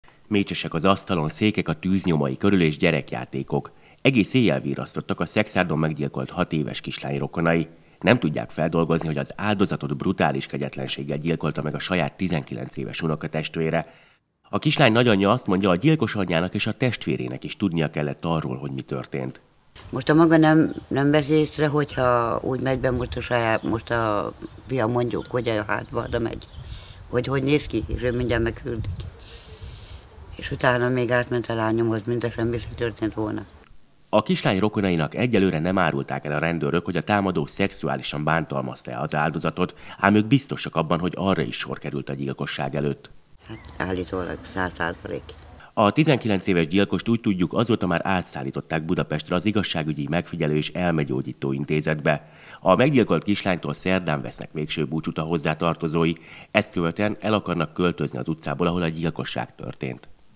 Megszólal a 6 éves meggyilkolt szekszárdi kislány nagyanyja 20190318
Megszólal a 6 éves meggyilkolt szekszárdi  kislány nagyanyja